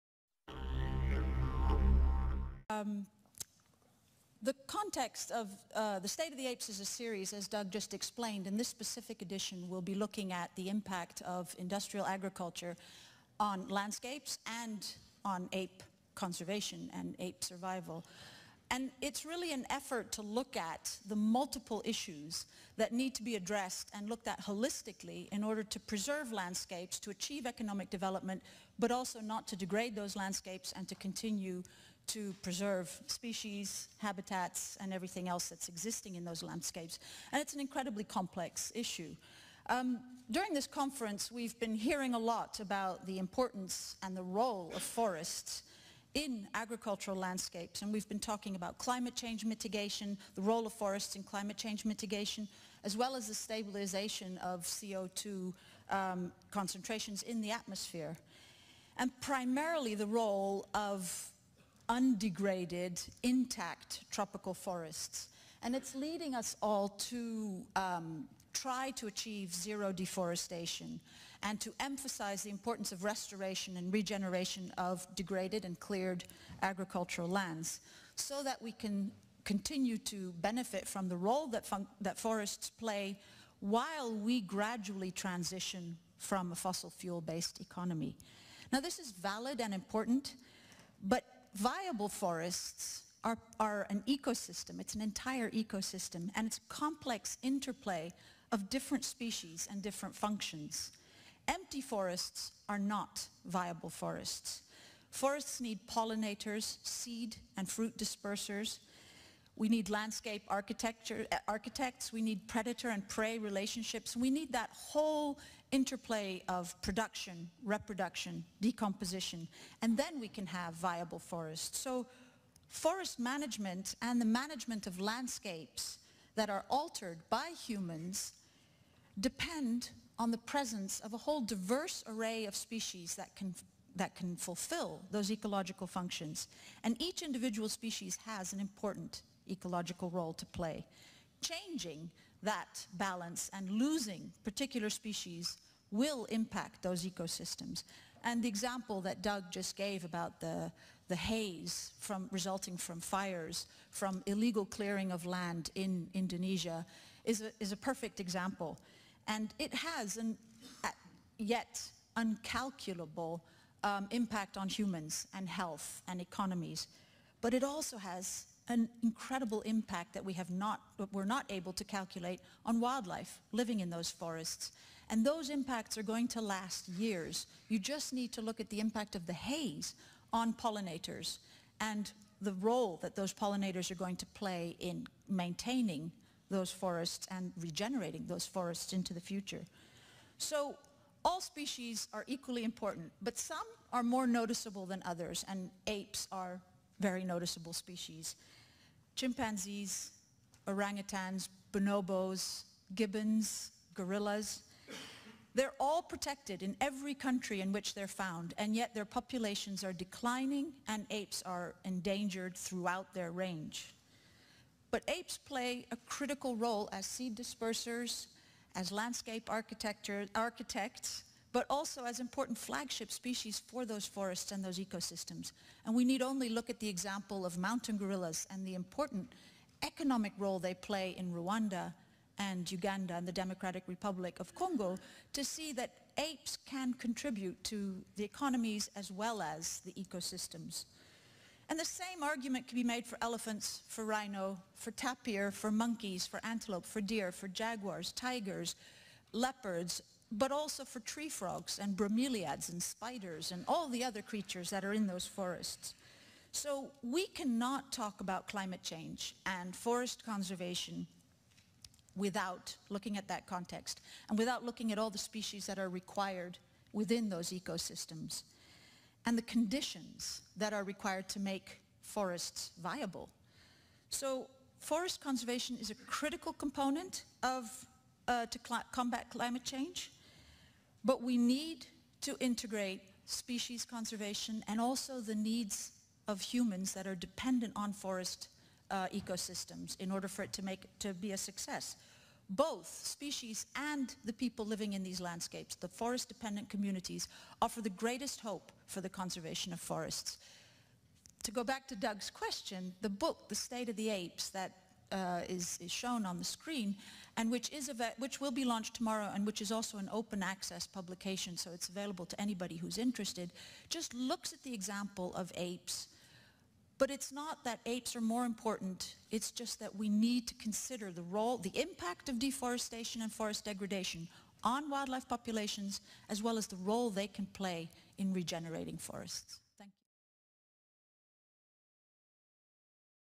State of the Apes 2: Arcus Foundation vice-president Annette Lanjouw
Arcus Foundation vice-president Annette Lanjouw addresses the issue of development and its impact on apes in Africa and Asia during the Global Landscapes Forum session, "Industrial Agriculture, Ape Conservation and Climate Change: More than a Business Case for Reducing Deforestation," which was chaired by GRASP coordinator Doug Cress